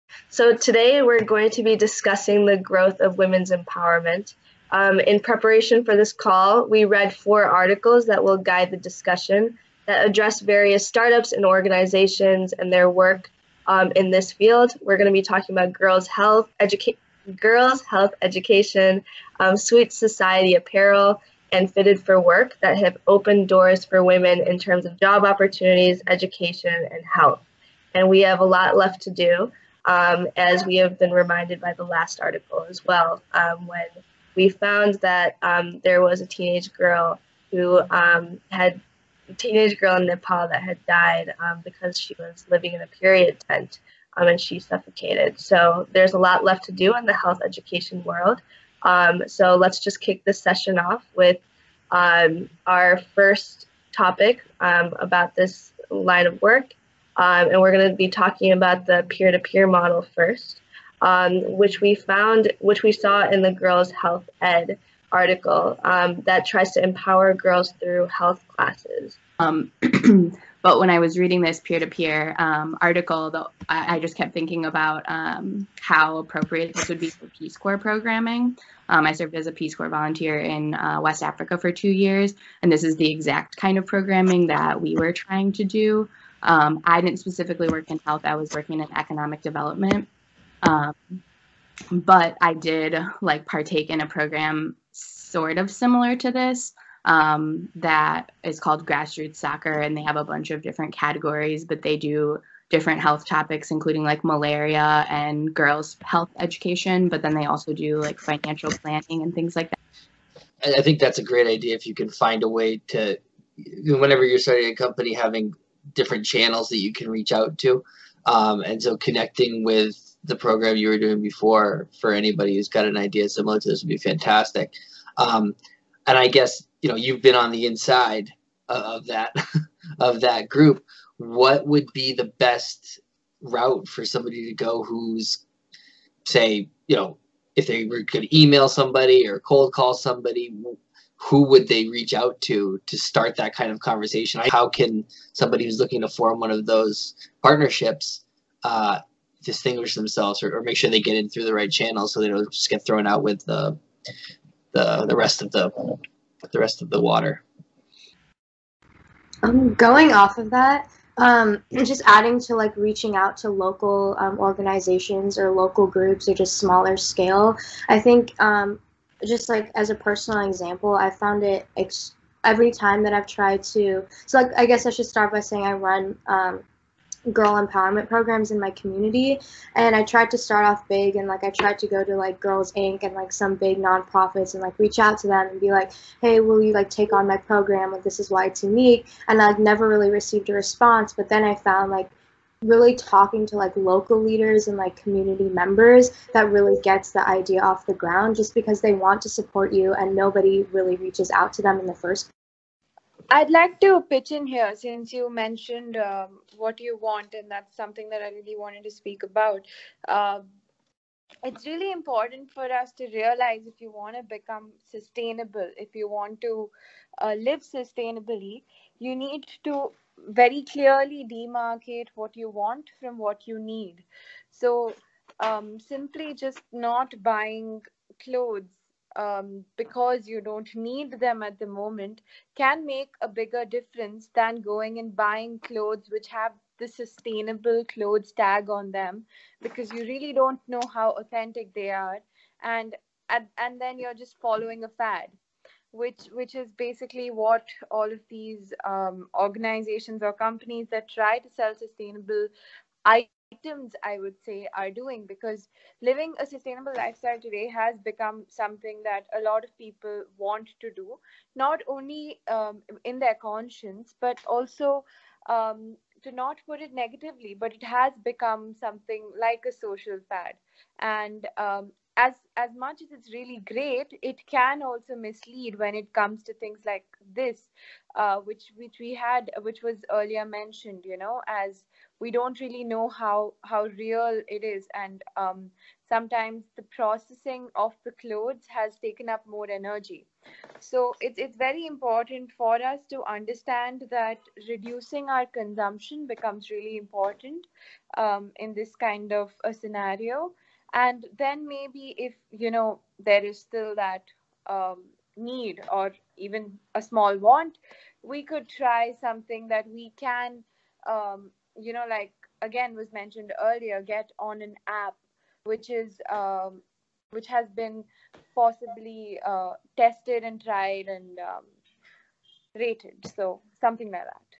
On empowering women through development I Impaction I Video Session 10 I 5 Minute Teaser
Impaction Innovators discuss the importance of women’s empowerment. Amongst other areas of discussion, the Innovators noted that peer-to-peer models can help improve education and international development efforts, while current sustainable fashion standards and marketing strategies can result in negative consequences in many countries at large.